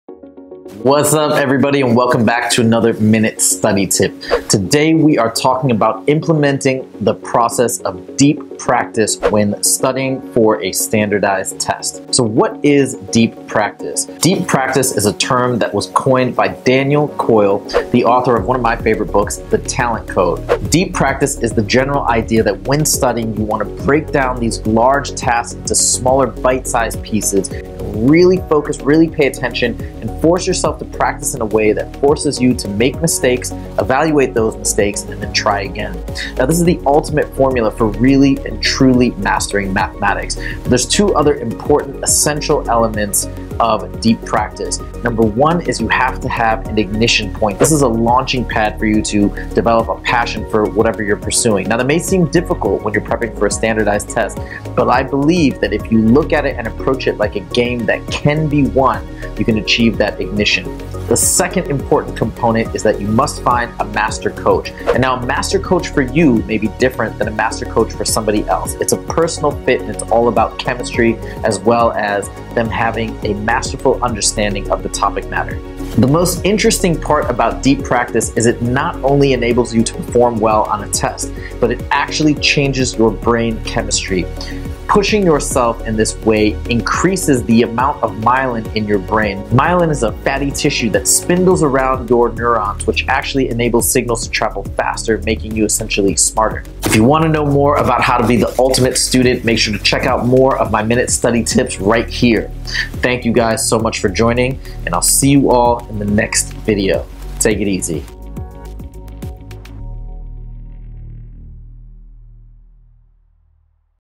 Deep Practice: the Key to Mastering Math for Standardized Tests - Minute Study Tip #10
The process of Deep Practice is how amateurs become masters. To learn the ins and outs of how to apply deep practice to standardized tests and mathematics generally, check out the full video. For more clutch study tips, tune in every Friday!